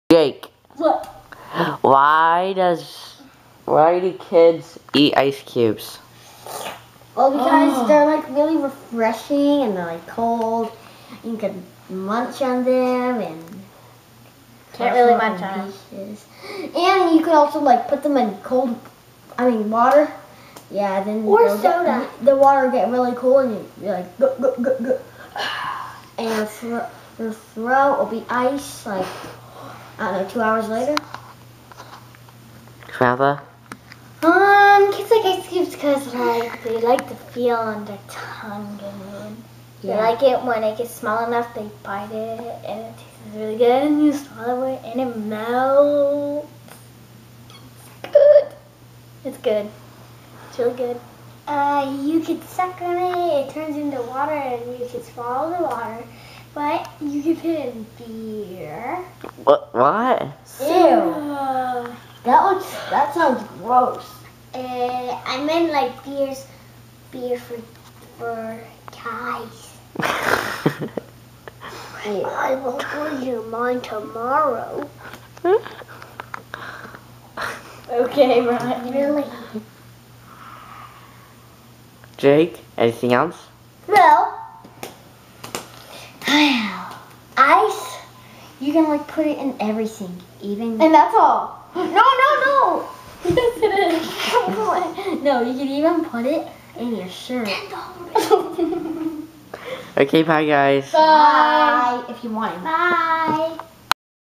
Why Does My Child Eat Ice Cubes?
Does your child love ice? Here is why!